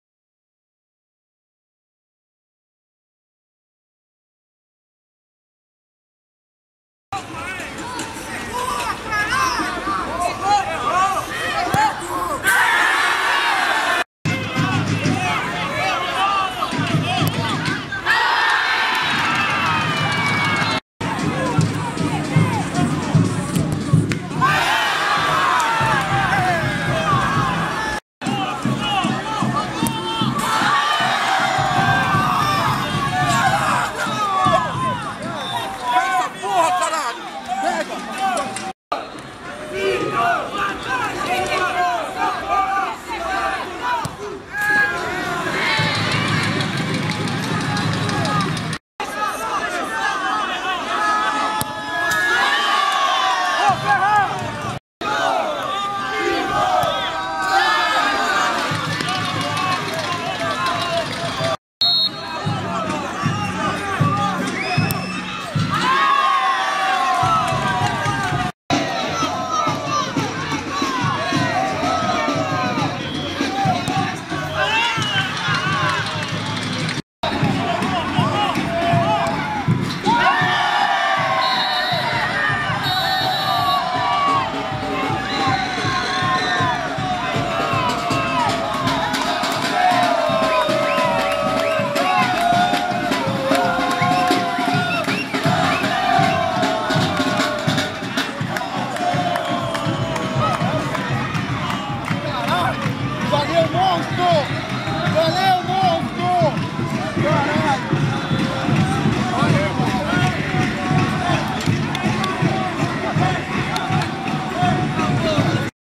NOVE DE JULHO X FAVELA DO HELIÓPOLIS
DISPUTA DE PÊNALTIS NOVE DE JULHO X FAVELA DO HELIÓPOLIS - QUARTAS DE FINAL SUPER COPA PIONEER - 12/03/2016